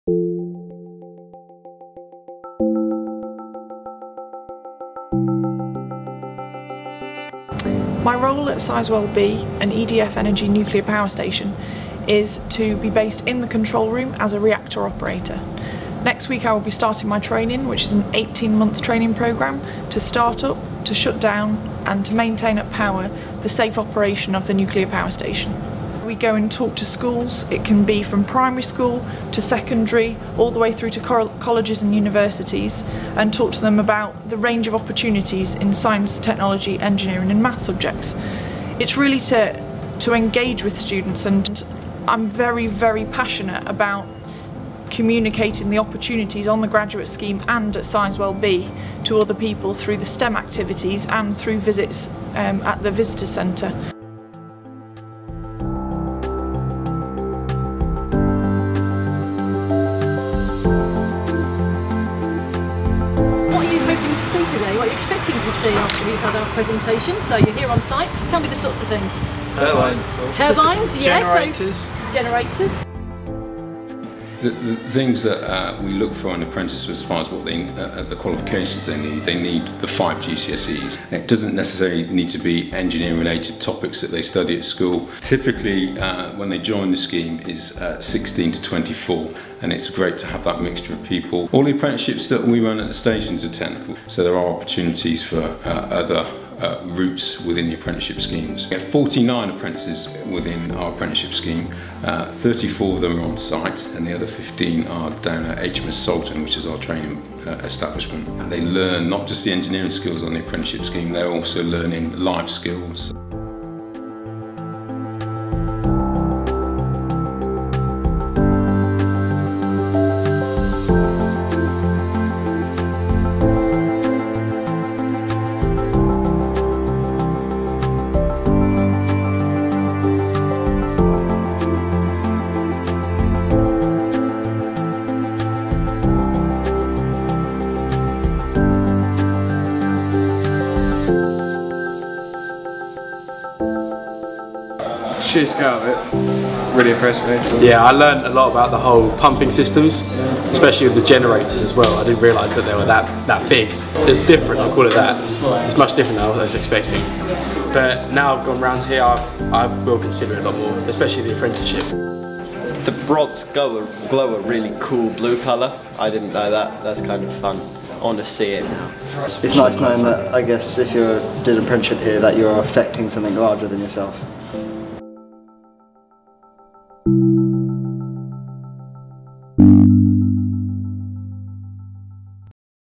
re:generation at Sizewell Visitors Centre (Full Version)
re:generation talks to Katie Bannister about her experience on the EDF Energy graduate scheme and her role as a STEM ambassador, and Roger Barge, Apprentice Co-ordinator at Sizewell B nuclear power station about what young people need to join the apprenticeship scheme as we follow students from the Colchester Institute on their tour of Sizewell.